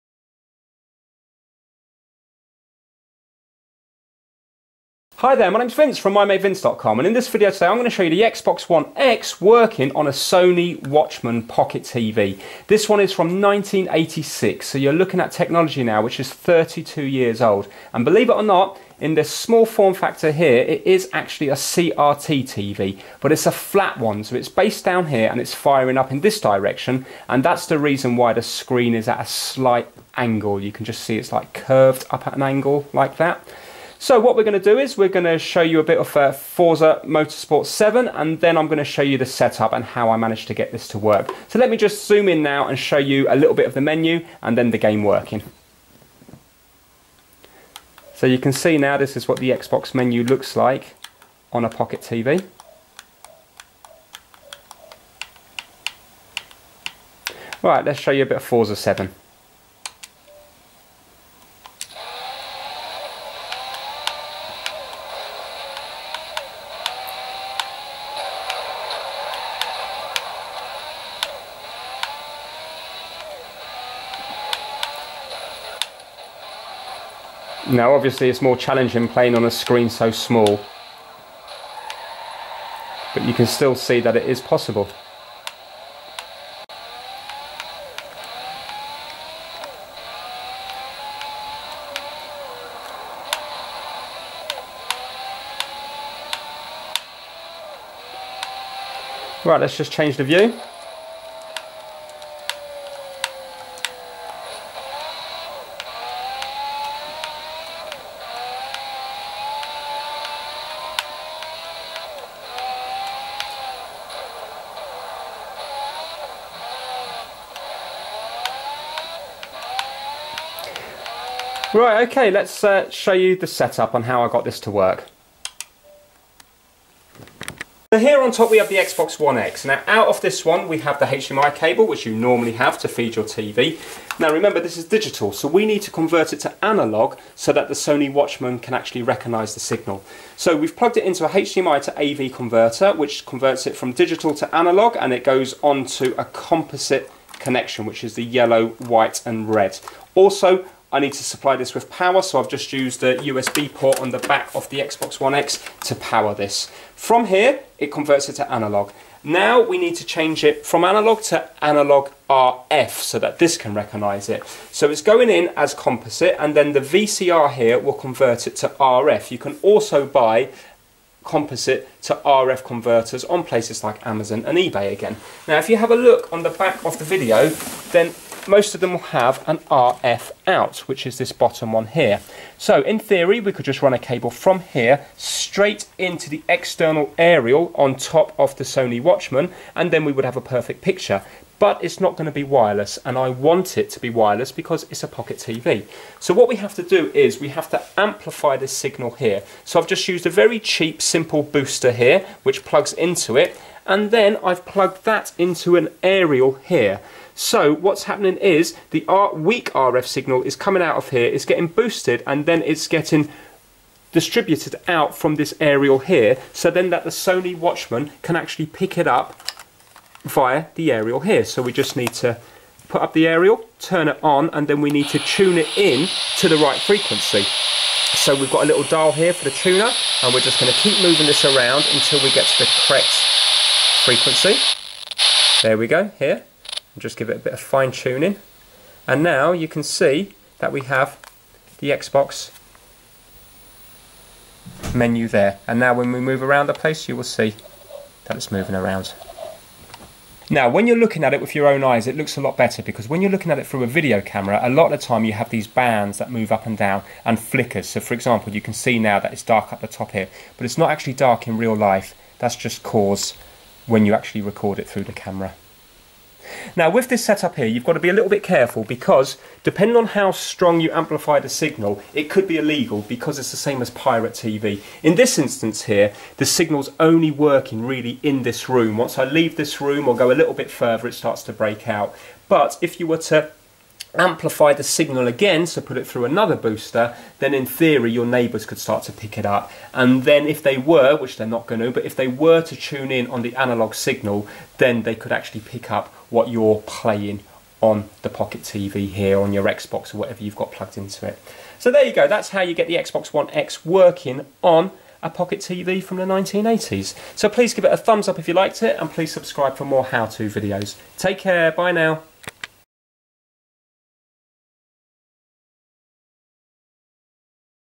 Xbox One X on a 1986 Sony Watchman CRT Pocket TV
Hi, this video shows you what a Xbox One X looks like on a 1986 Sony Watchman Pocket Television. The TV has a black & white CRT screen. I also show you how to set it up.